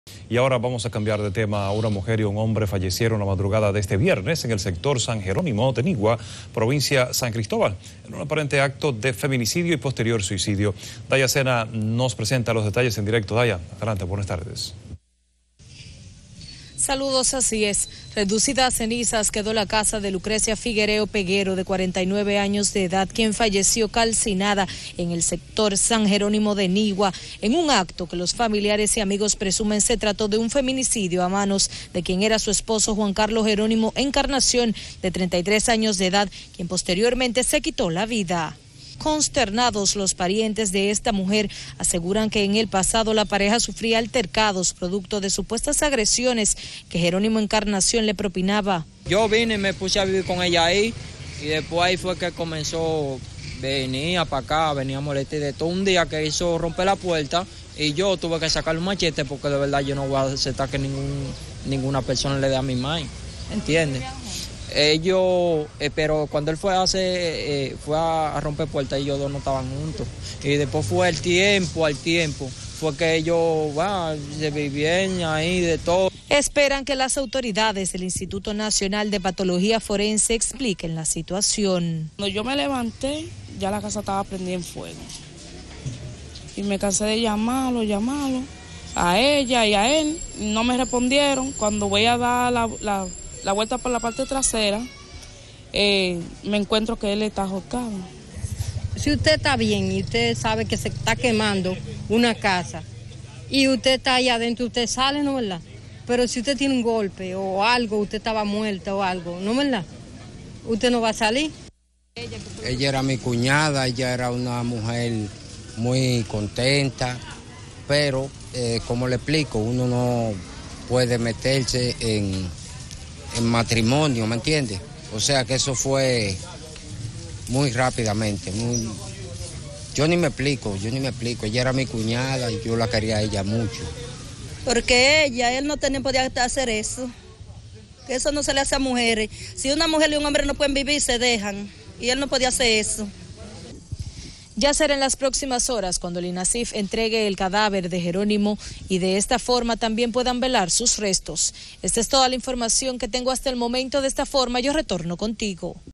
Muere pareja en Nigua en aparente feminicidio-suicidio
Nigua, RD.- Una mujer y un hombre fallecieron la madrugada de este viernes en el sector San Gerónimo de Nigua, provincia San Cristóbal, en un aparente acto de feminicidio y posterior suicidio.

Informaron que Lucrecia Figuereo Peguero, de 49 años de edad, falleció calcinada al quedar su residencia reducida a cenizas, en un acto que familiares y amigos presumen se trató de un feminicidio por su esposo Juan Carlos Gerónimo Encarnación, de 33 años de edad, quién luego se quitó la vida